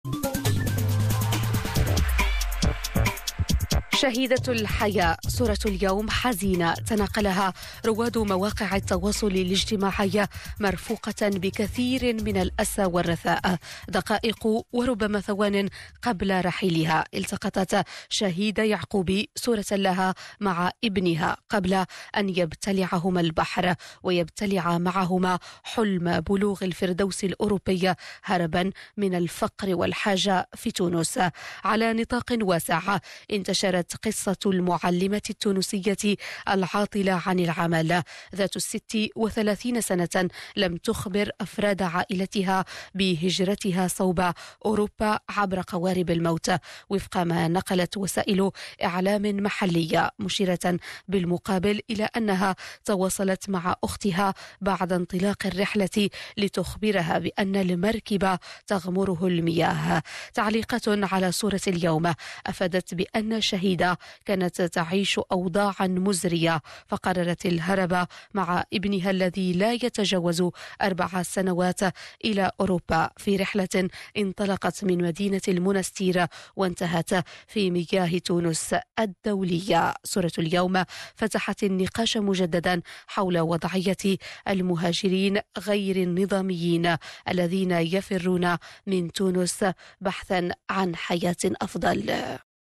سيلفي أخير قبل الرحيل
اسمها كان جزءا من حكايتها الحزينة، شهيدة_يعقوبي تونسية لقت حتفها على مركب للهجرة غير النظامية. سيلفي التقطته قبل الرحيل انتشر على مواقع_التواصل_الاجتماعي ، وتنقل تفاصيل حكايته مريم_مرغيش في الصورة على ميدي1 .